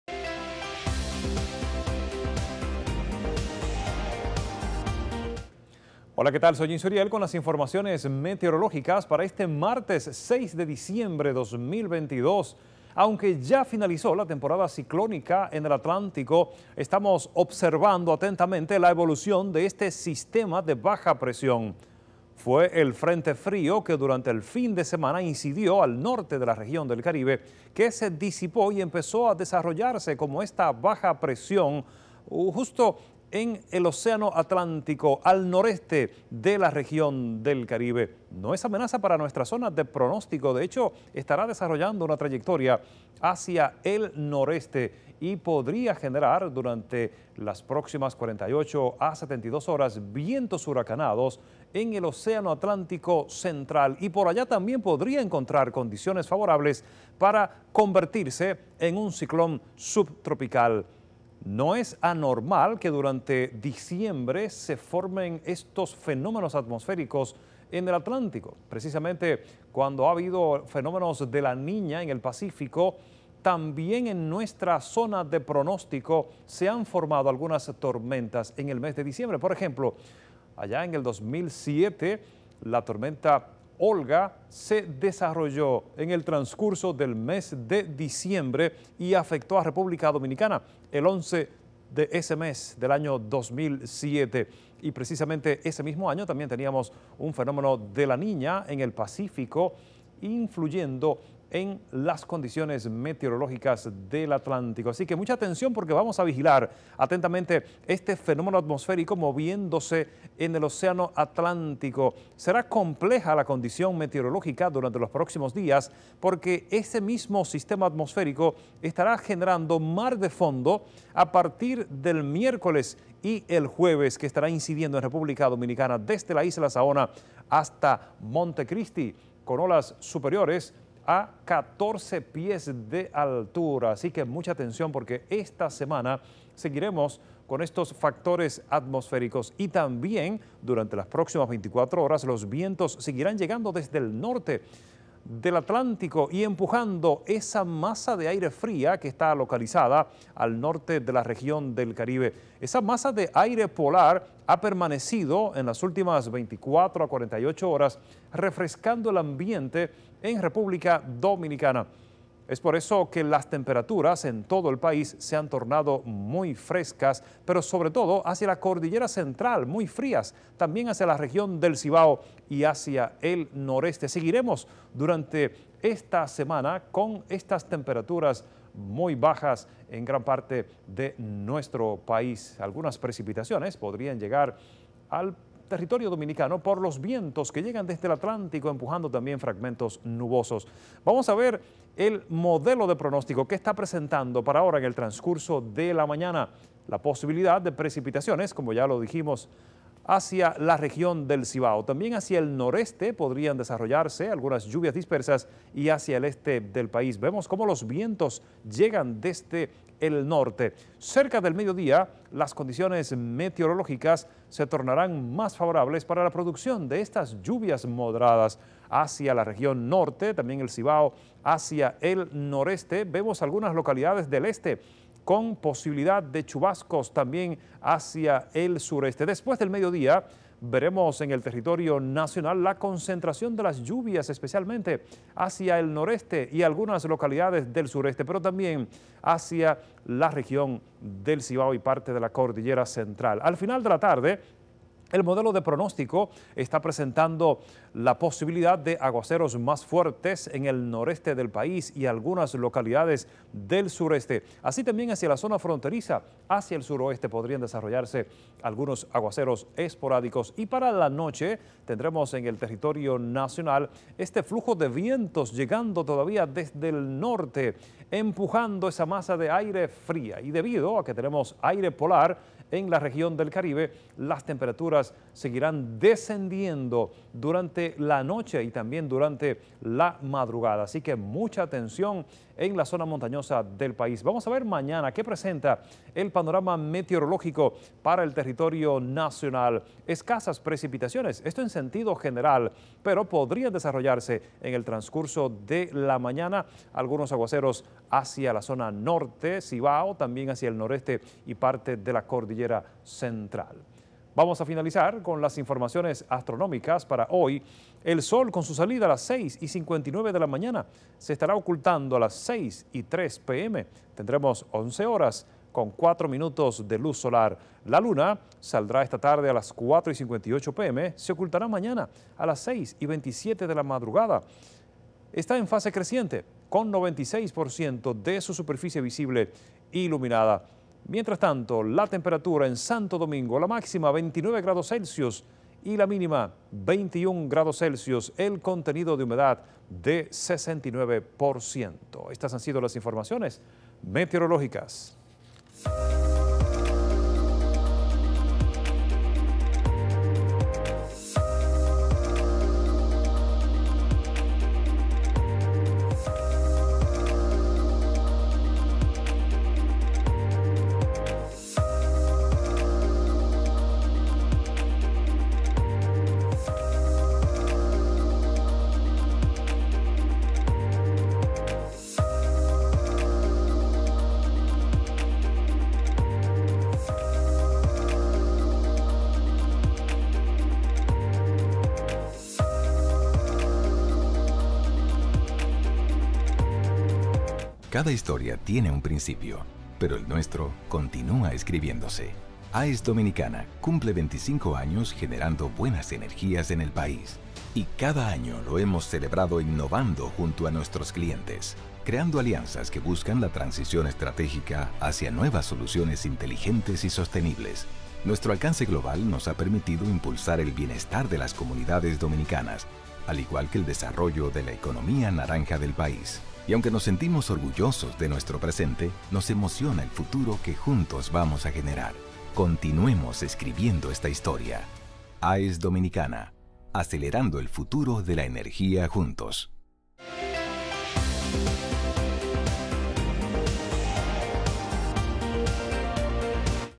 Martes 6 diciembre | Posible desarrollo ciclónico al noreste del Caribe; temperaturas frías en RD
Estamos vigilando un sistema de baja presión en el Atlántico con potencial de convertirse en un ciclón subtropical en los próximos días. No es anormal que haya actividad ciclónica en diciembre: hablamos de la tormenta Olga del 2007. Mientras que el aire polar seguirá incidiendo en el Caribe.

ACTIVA LAS NOTIFICACIONES PARA RECIBIR EL PRONÓSTICO DIARIO